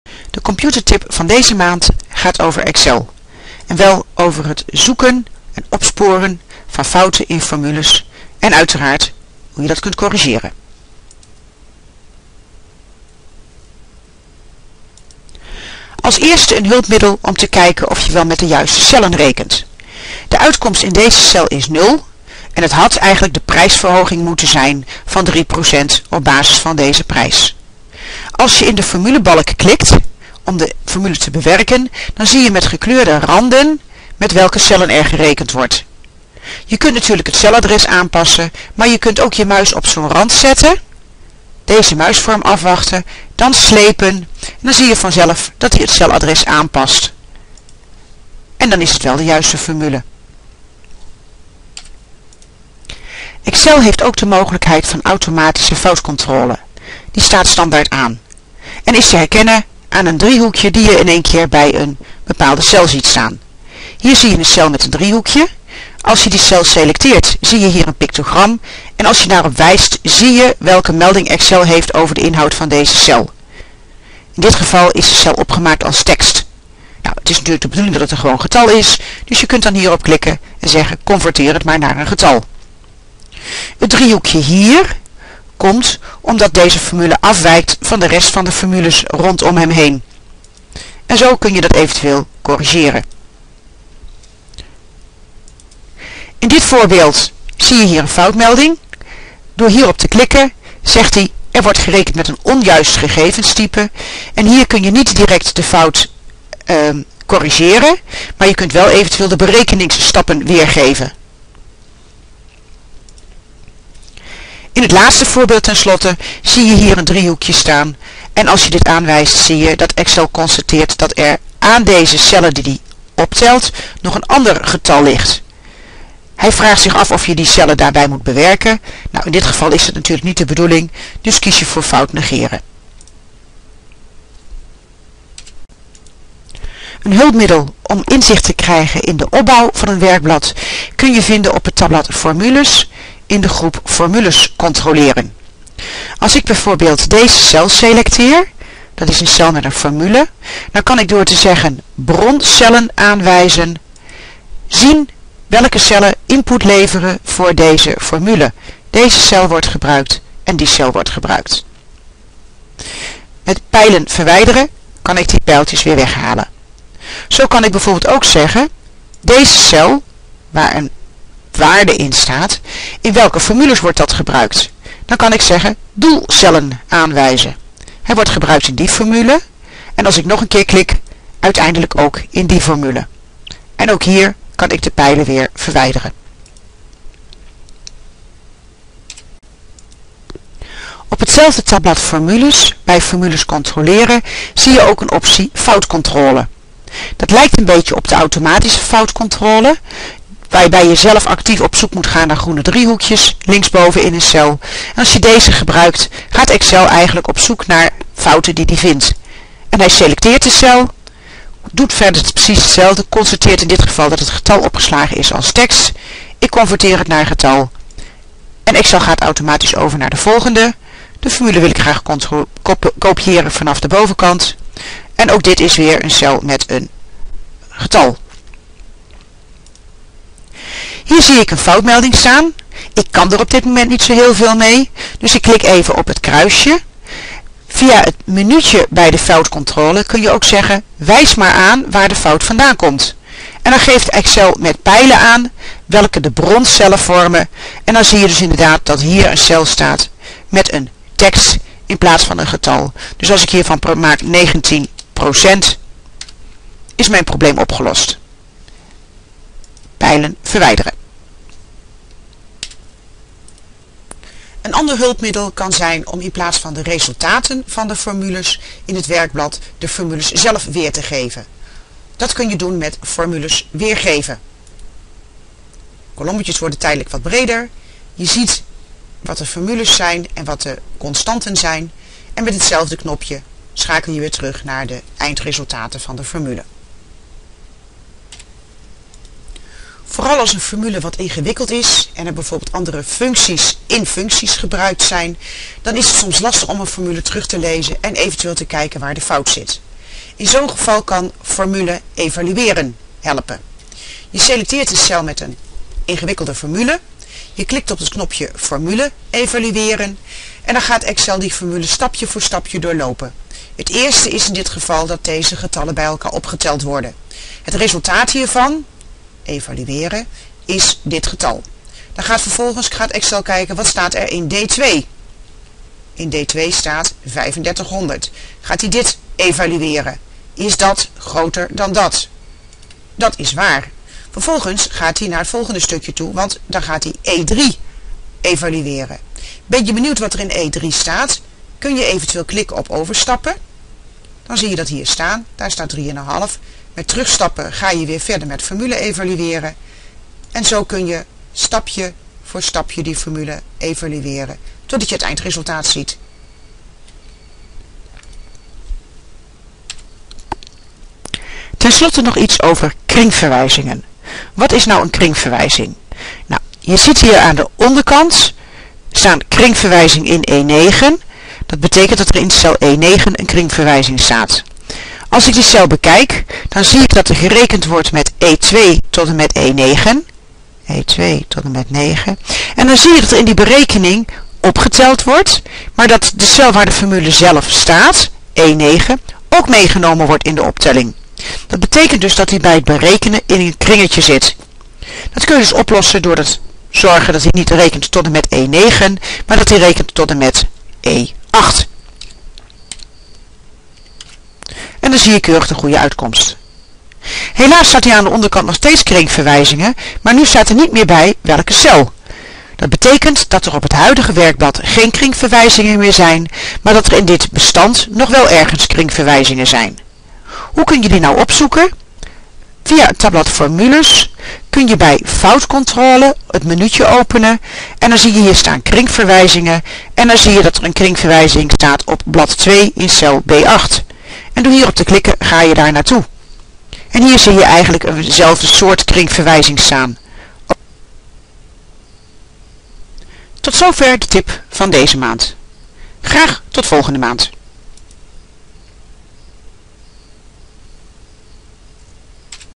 Formules controleren in Excel
Excel heeft enkele handige hulpmiddelen om fouten op te sporen in formules. In deze video zie je de mogelijkheden.